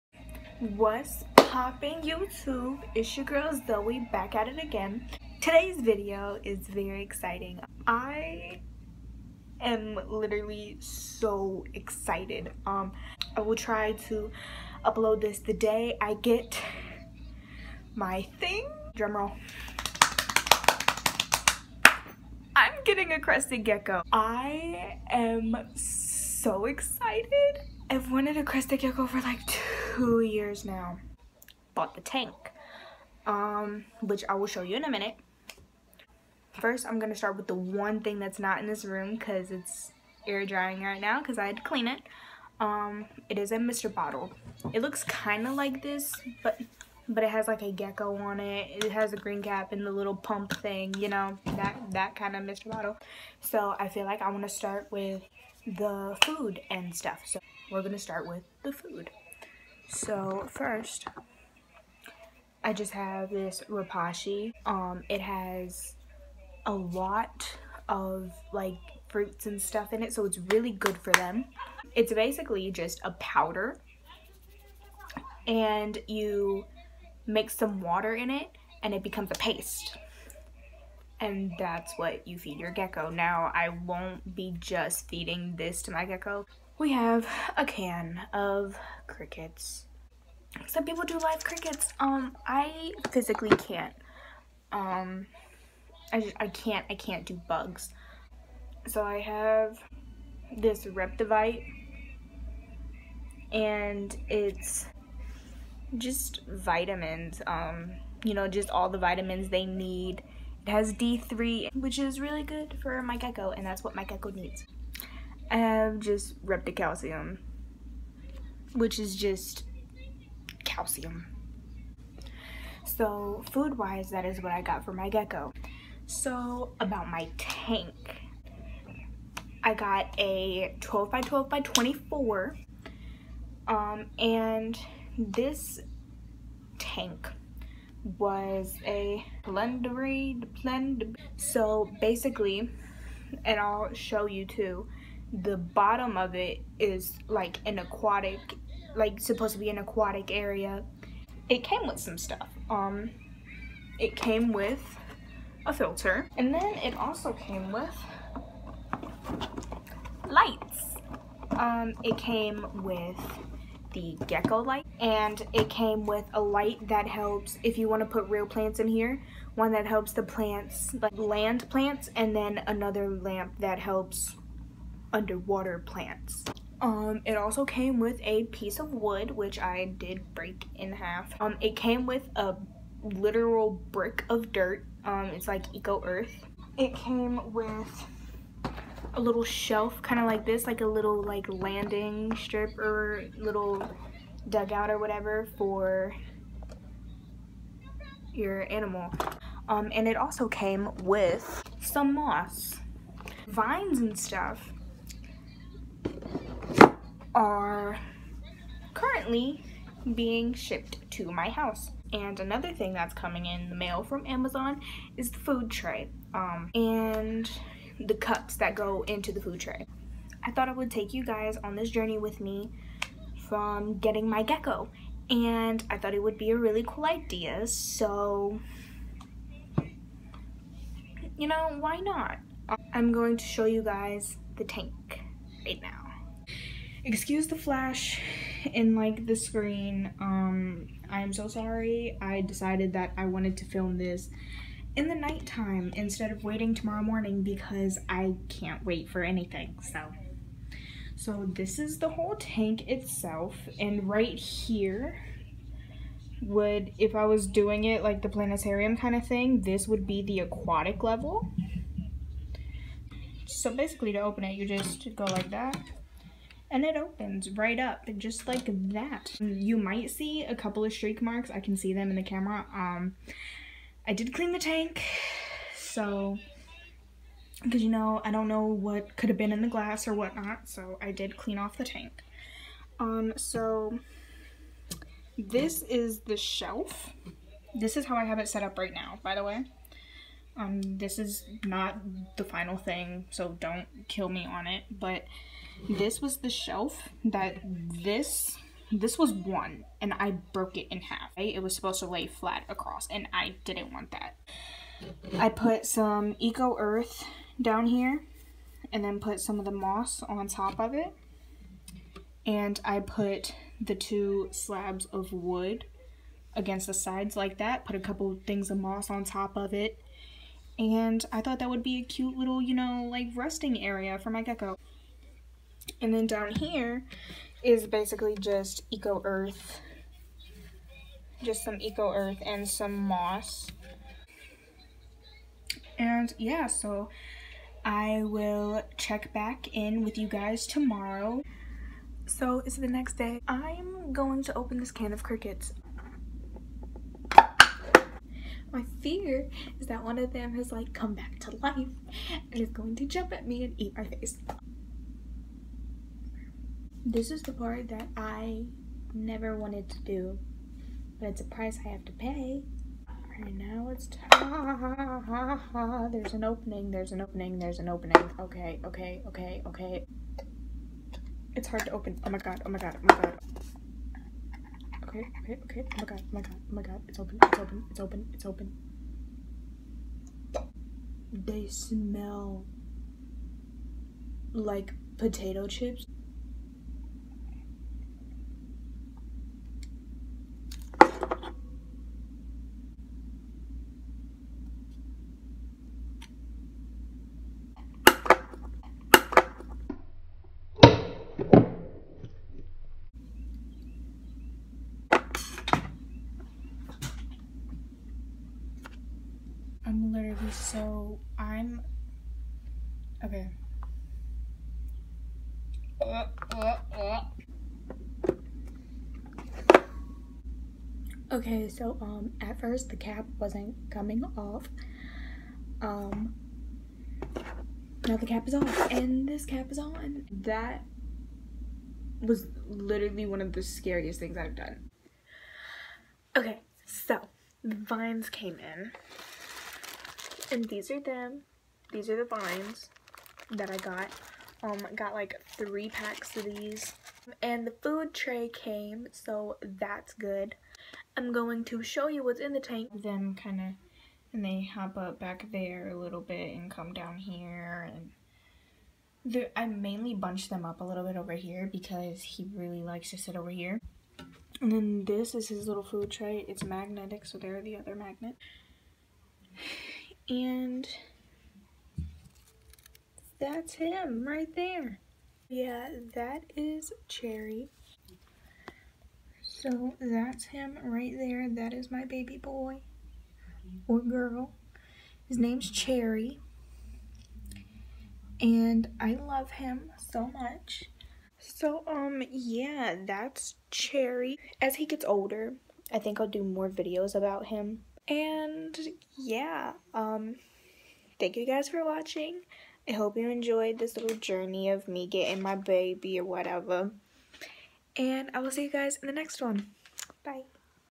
I Got A Crested Gecko!!!
food/calcium i got at petco
tank and gecko i got at petsmart
vines and food tray i got from amazon